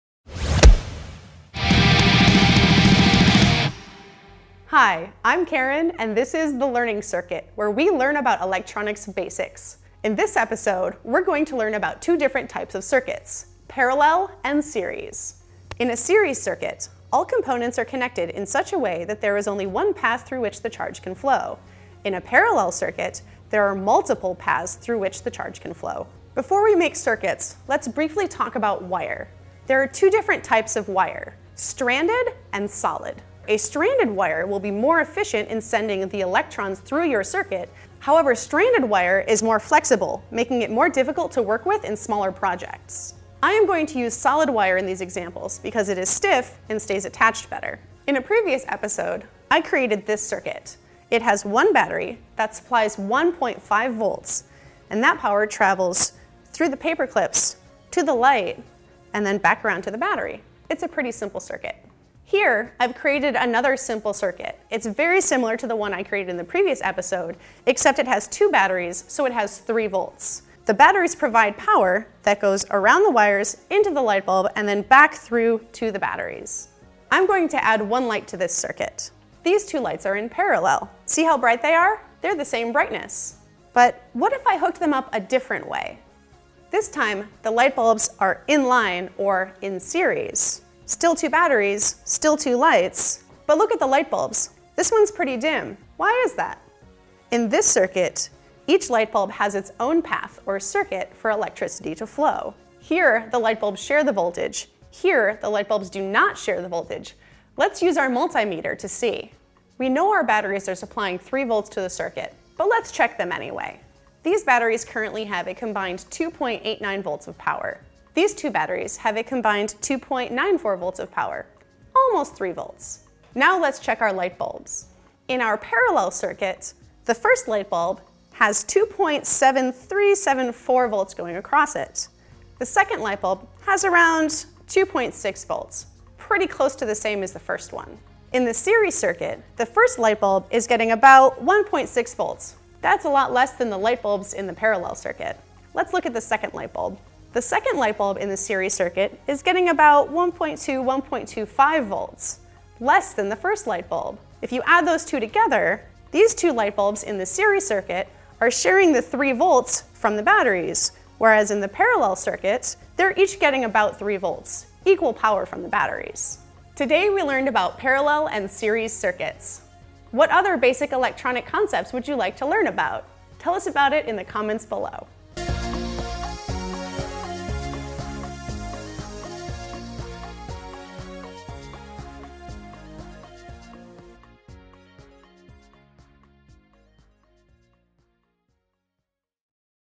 Series & Parallel Circuits - The Learning Circuit
Karen demonstrates the difference between parallel and series circuits by adding additional voltage and another bulb to a simple circuit.

Simple circuits such as the ones Karen demonstrates are an easy way to learn about the difference between simple and parallel circuits. In a Series Circuit, all components are connected end-to-end to form a single path for electrons to flow.  In a Parallel circuit, all components are connected across each other to form two sets of electrically common points.

Another consideration you’ll need to understand when getting started with electronics projects is your choice between using stranded or solid wires.  Both types of wiring are effective means of transmitting electricity, however, a stranded wire is more efficient in sending the electrons through your circuit.  It’s also better suited for more intricate usages such electronic devices and circuit board where it offers the flexibility to be bent or twisted to connect electronic components.  Solid wire is frequently used for outdoor or rugged applications where wire may be exposed to corrosive elements, adverse weather conditions, or frequent movement. It’s also a good choice for smaller projects where the flexibility of stranded wire makes it difficult to use.

Karen uses solid wire to demonstrate the difference between series and parallel circuits because it’s stiff and stays attached better.  In electronics, a node refers to any point on a circuit where two or more circuit elements meet. In a schematic, the node will be the wires between the components.  In a circuit the current flows from high voltage to a lower voltage. The current flows through every path it can take to the point of lowest voltage (commonly referred to as ground).  Two components are said to be in series if they share a common node and the same current flows through them.  If the components share two common nodes then they are said to be in parallel.